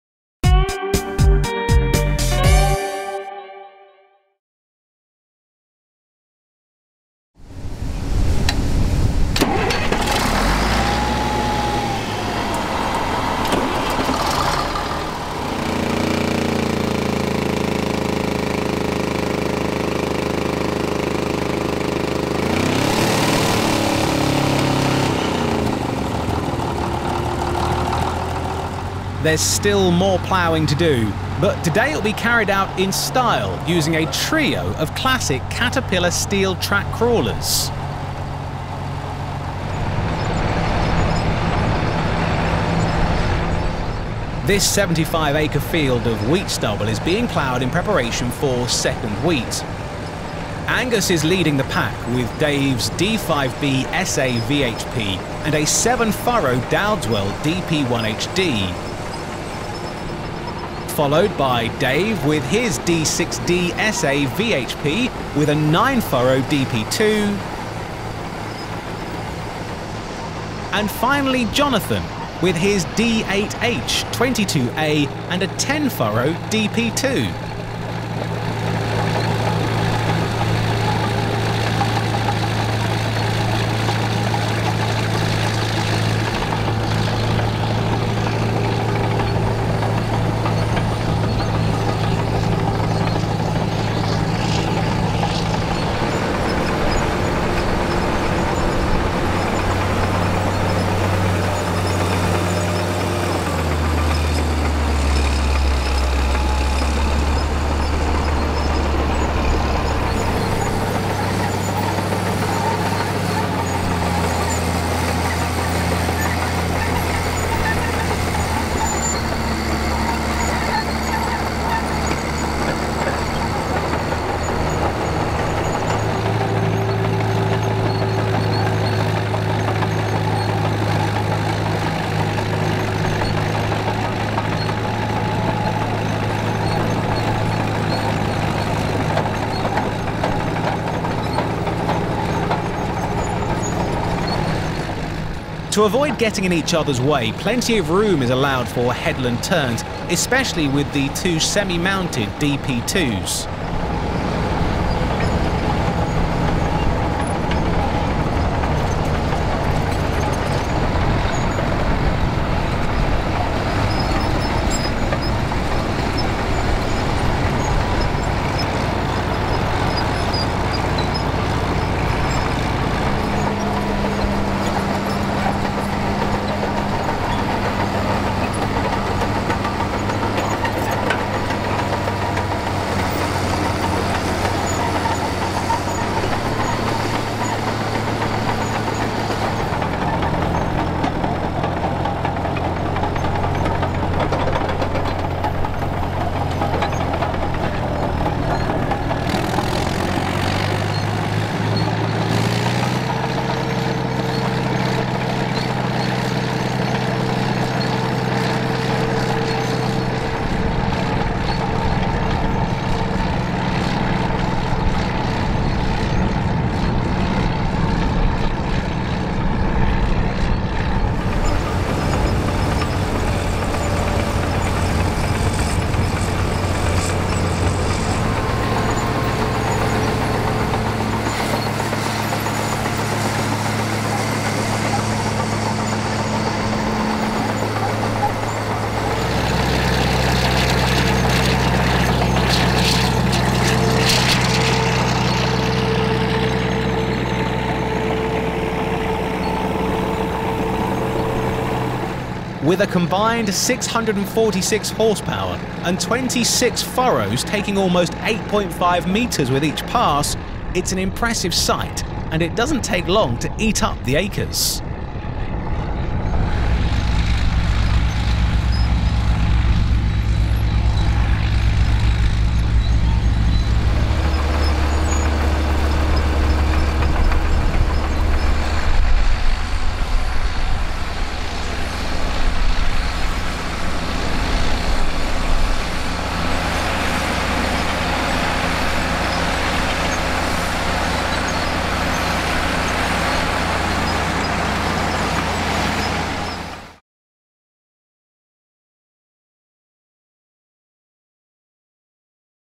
Caterpillar D5B SA VHP, D6D SA VHP and D8H 22A ploughing | Dowdeswell DP1 | 26 furrows | Classic Cat
Ploughing with three classic steel track Caterpillar crawlers, all working with Dowdeswell ploughs, with a combined 646hp pulling 26 furrows. Featuring a Caterpillar D5B SA VHP and 7f Dowdeswell DP1 HD, Caterpillar D6D SA VHP and 9f Dowdeswell DP2 and Caterpillar D8H 22A and 10f Dowdeswell DP2.
This fantastic sight of multiple Cats working together is one of the highlights of Part 2 of our Project 20 DVD series, to find out more please